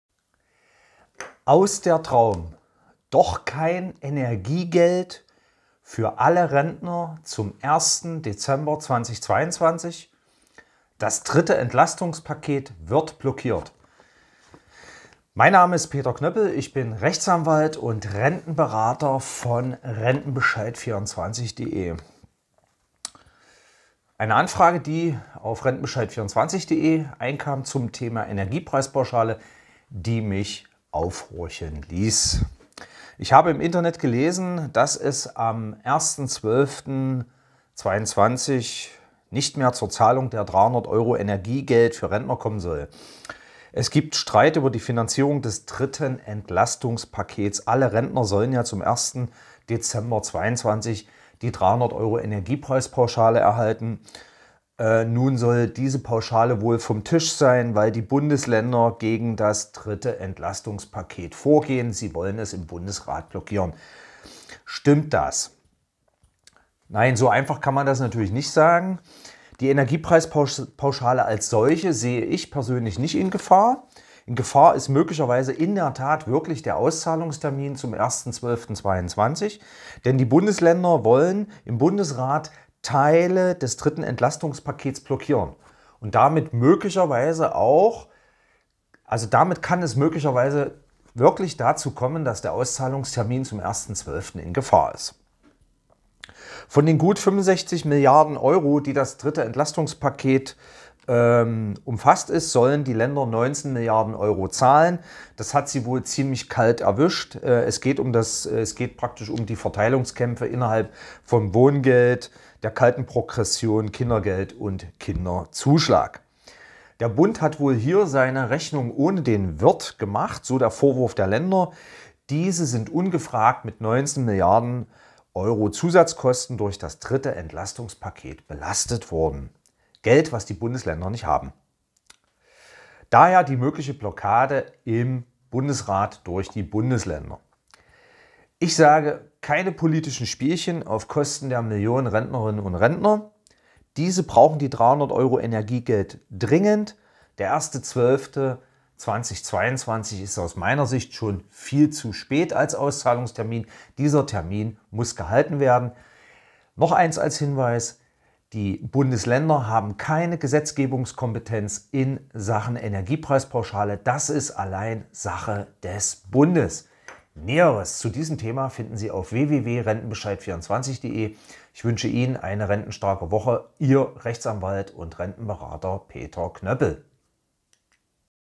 Aus der Traum: Kein Energiegeld für Rentner zum 01.12.22? Zoff um das Entlastungspaket
Kommt es doch nicht zur Auszahlung der 300€ Energiepauschale an 21 Millionen Rentnerinnen und Rentner am 01.12.22. Bundesländer und der Bund streiten sich über die Kosten des dritten Entlastungspaketes. Ein Streit der zu Lasten der Rentner geht. Zahlungstermin in Gefahr. Dies muss nicht sein!